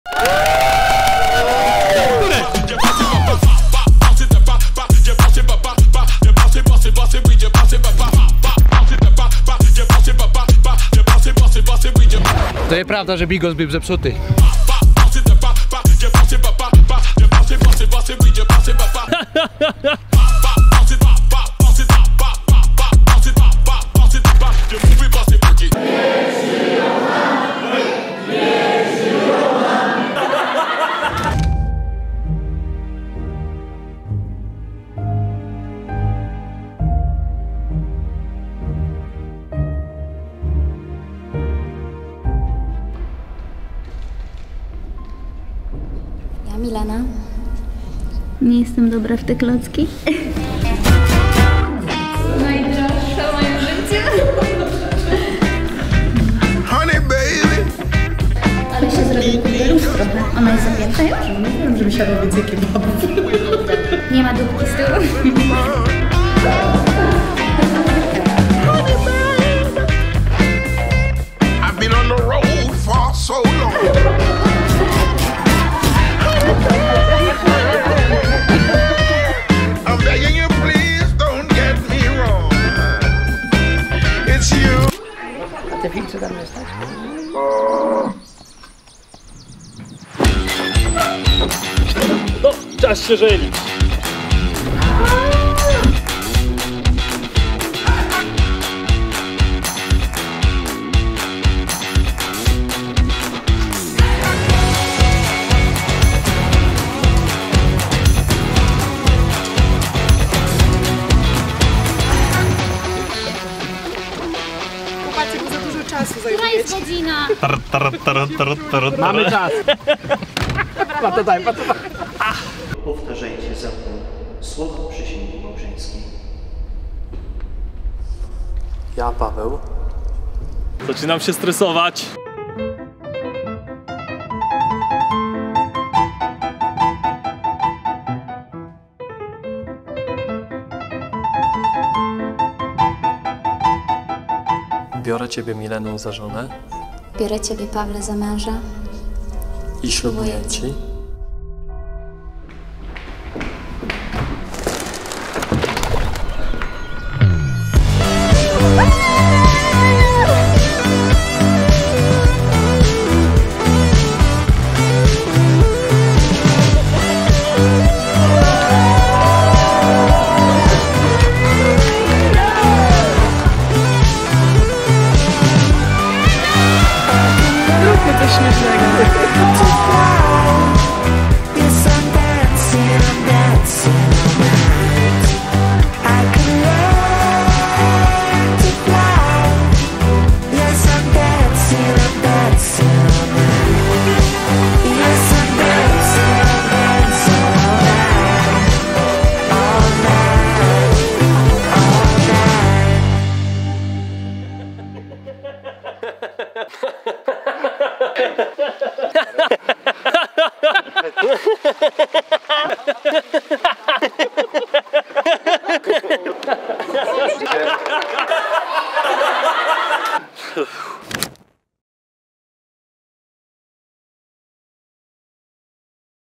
Szalony film ślubny | HA HA HA
Paweł i Milena dali czadu! Ich ślub to była jedna, ogromna pigułka emocji. Masa śmiechu i pozytywnej energii ale nie tylko...